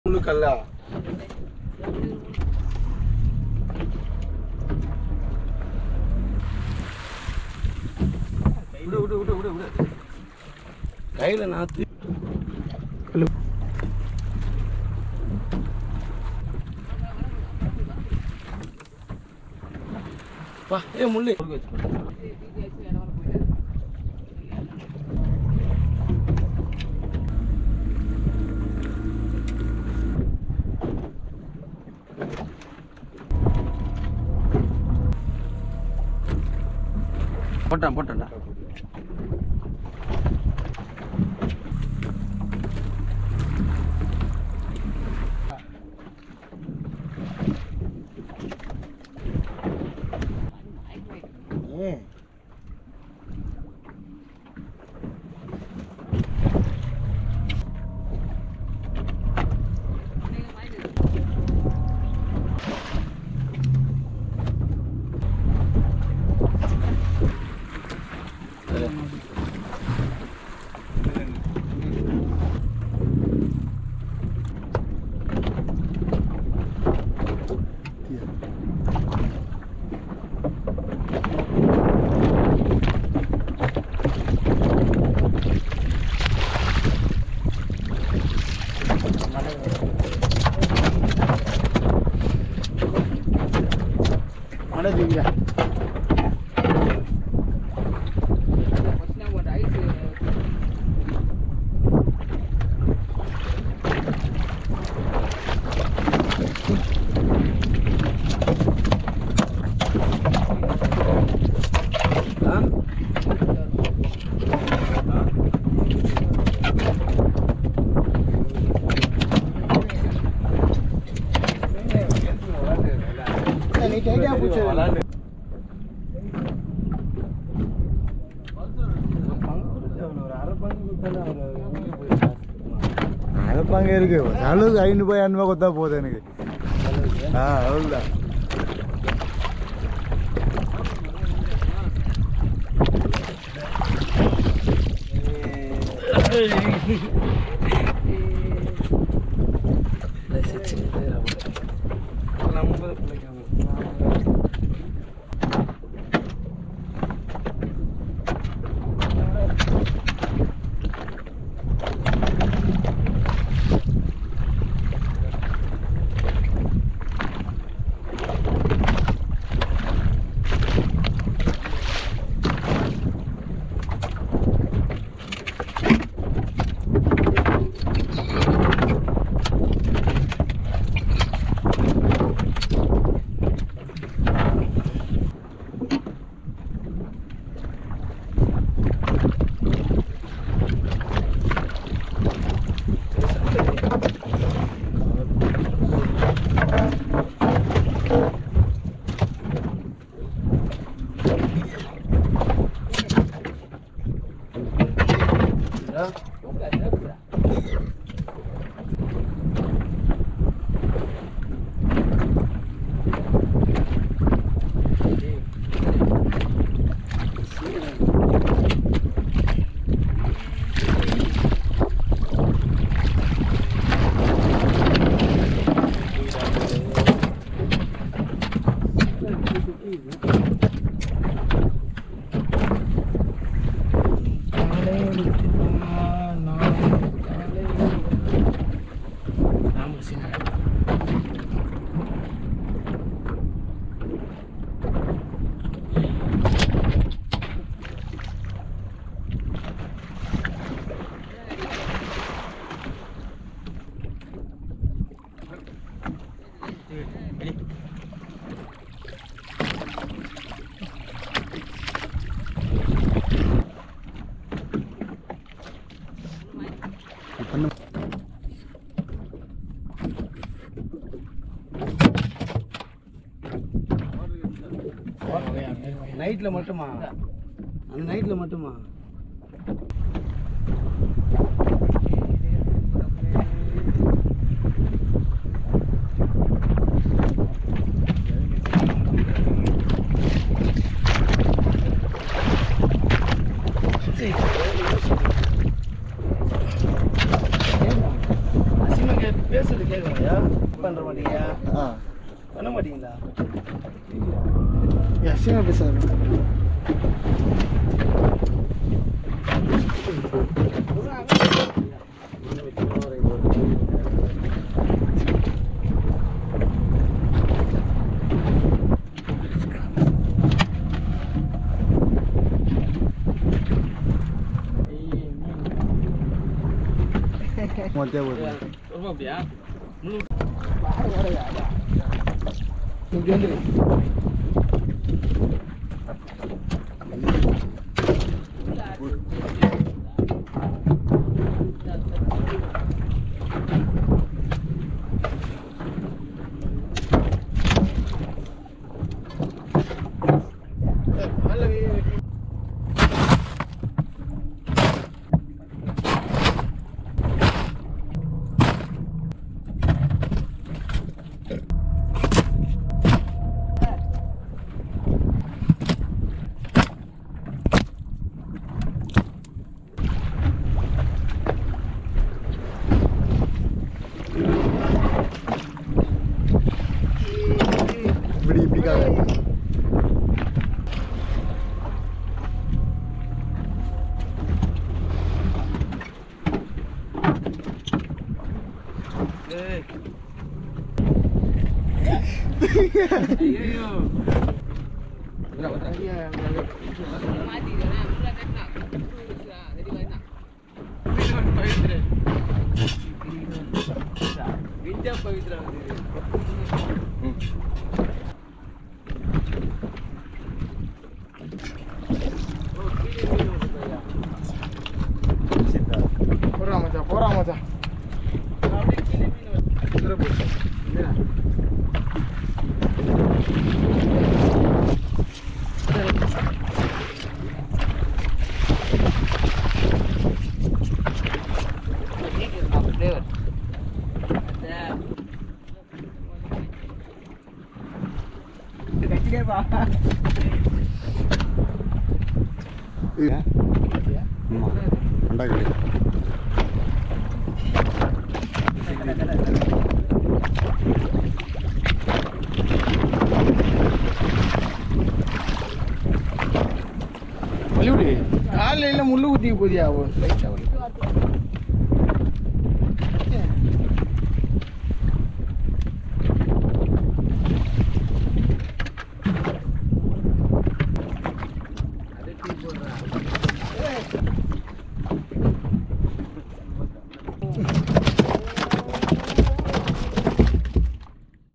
Catching Giant Trevally Fish & Marine Cat Fish in Deep Sea
We Caught Giant Trevally Fish & Marine Cat Fish in the Deep Sea